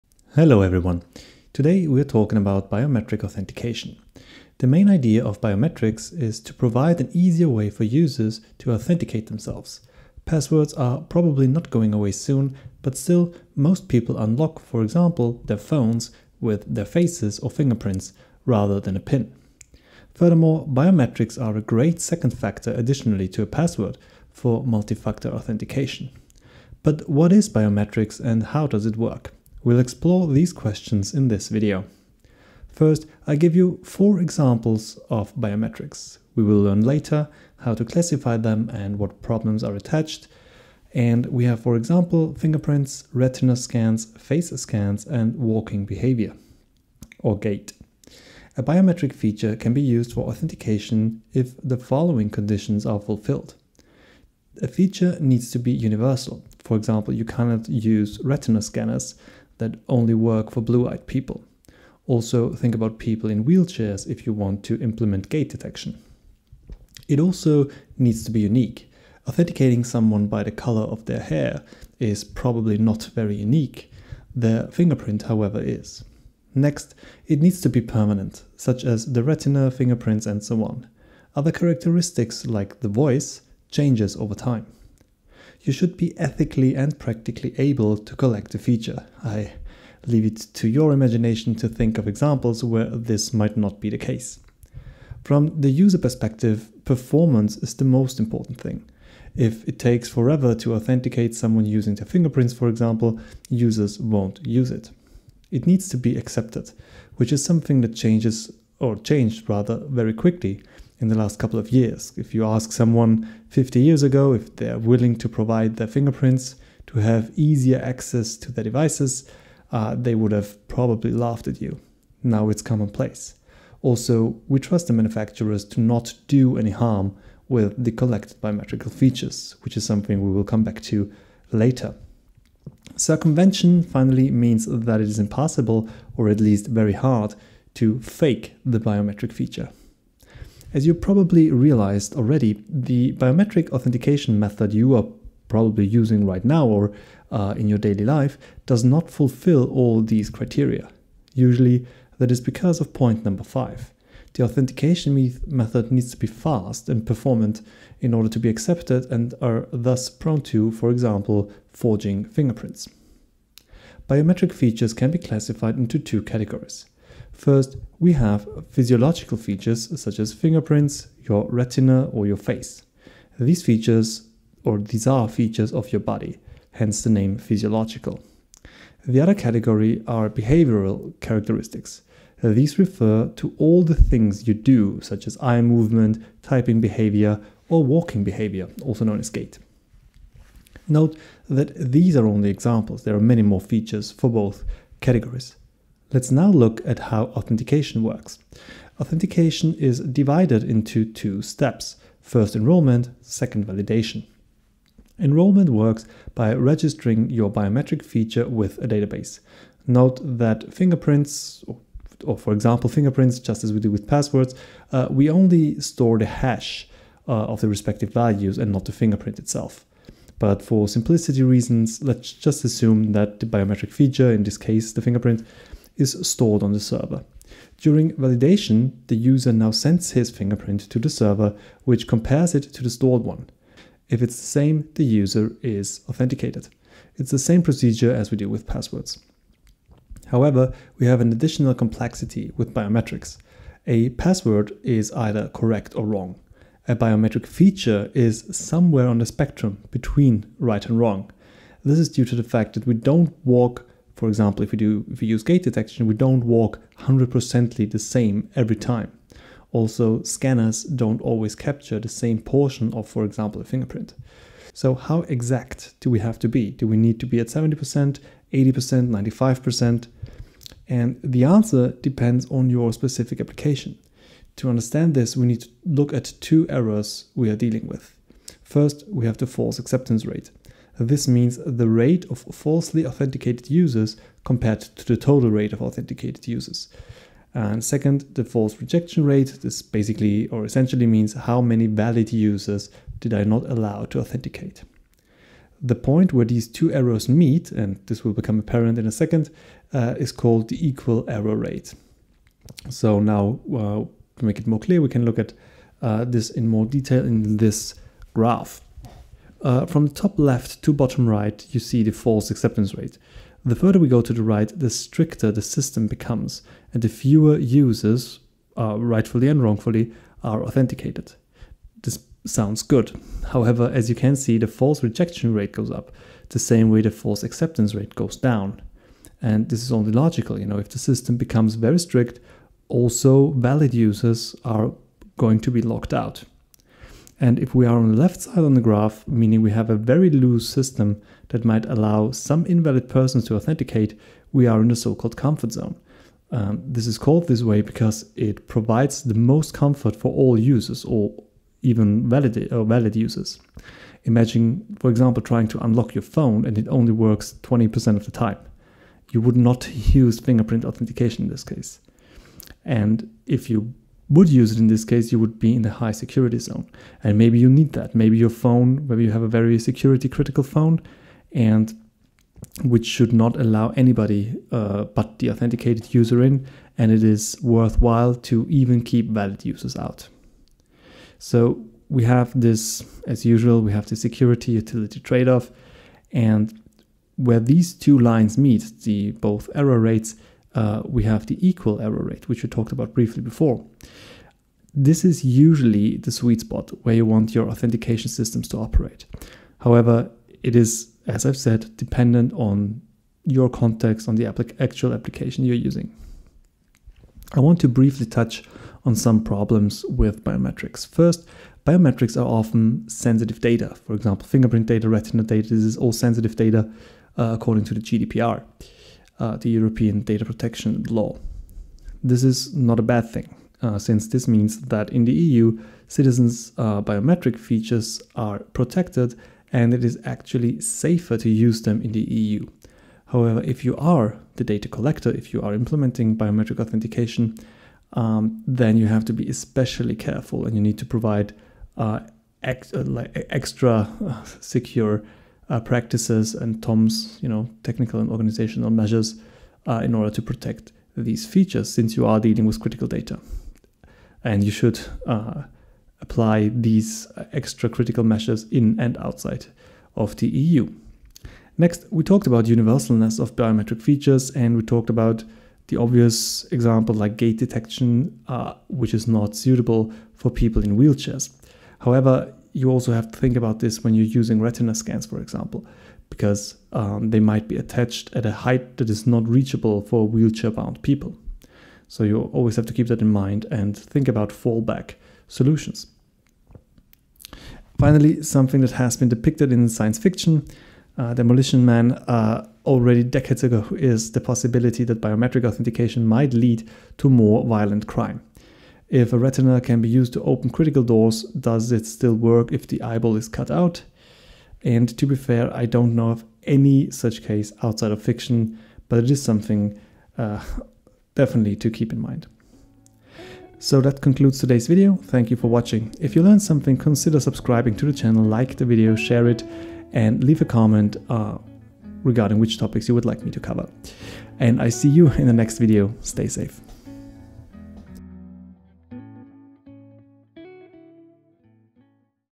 Biometric Authentication Explained
I go into detail about biometrics, biometric authentication, how to classify biometric features, and problems attached.